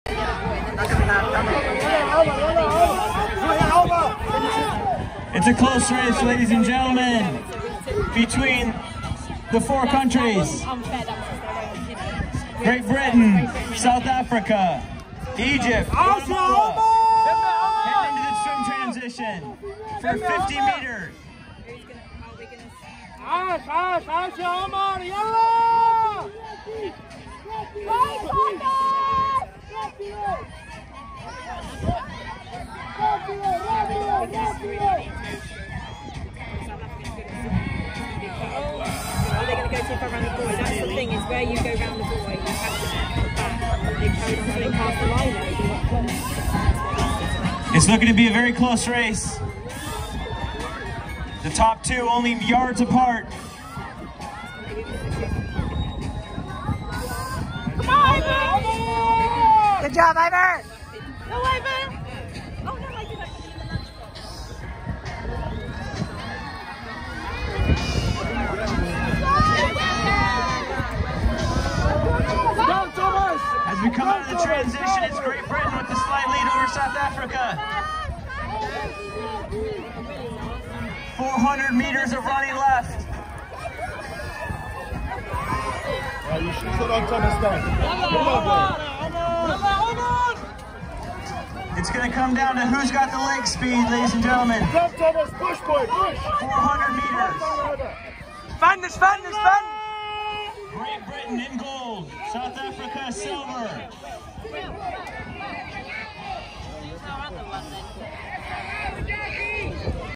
2019 UIPM Biathle / Triathle World Championships -- Biathle Mixed Relay Under 11 (Part 3)
Here's Iver Iverson of Wolfpack Pentathlon Club, at age 8, starting the swim leg of the Under 11 Division of the Biathle Mixed Relay.